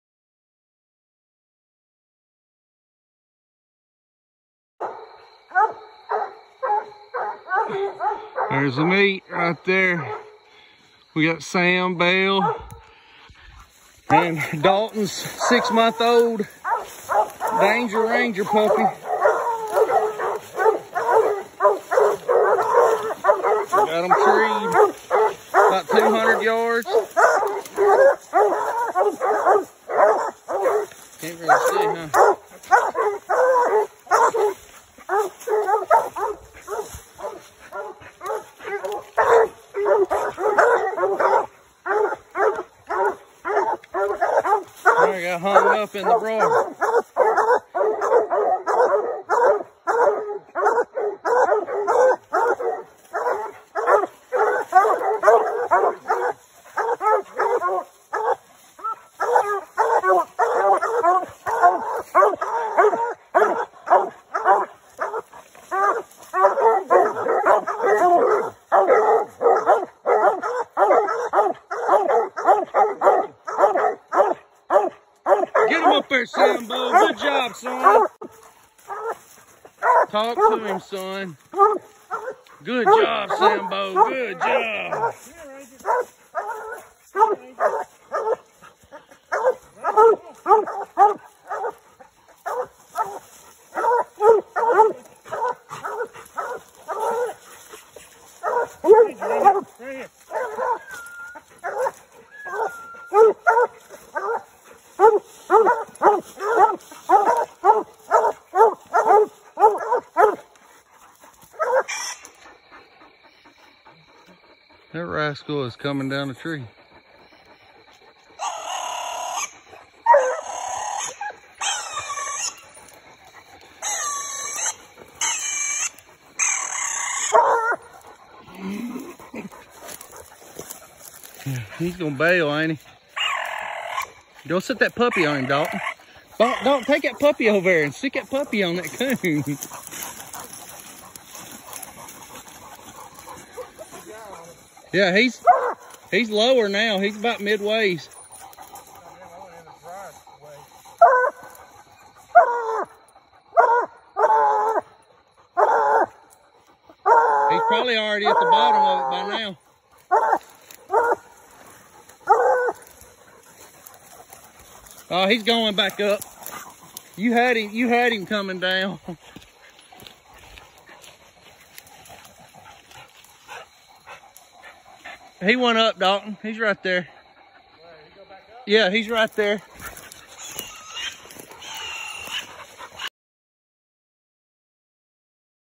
Coon hunting with Dalton and Belle
Coon hunting Walker hounds in Tennessee
Tree dogs mr clean hound track man hound